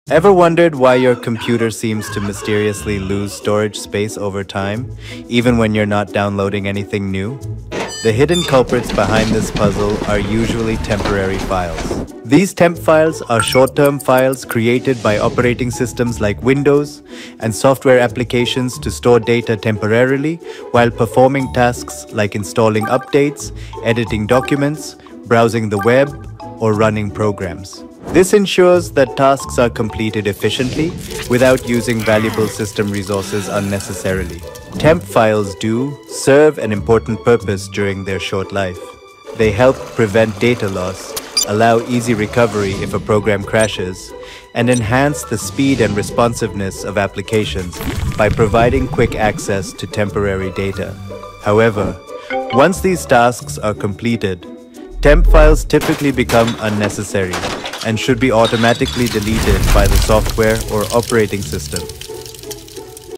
Storage Space Vanishing? Here’s how Temp Files are Secretly Filling Your Drive!
Is your computer running out of storage space for no apparent reason? Discover how temporary files are secretly filling up your hard drive and slowing down your system!

In this 3‑D animated Short we crack open the hidden world of temporary (temp) files— those behind‑the‑scenes scraps your Operating System and apps create to keep installs smooth, browsers snappy, and documents safe from sudden crashes.

In just one minute you’ll learn:

•What temp files are & why they exist?

•How they speed up tasks and guard against data loss?

•Why they quietly pile up and steal precious gigabytes?

Perfect for beginner tech enthusiasts who want faster devices and simpler explanation.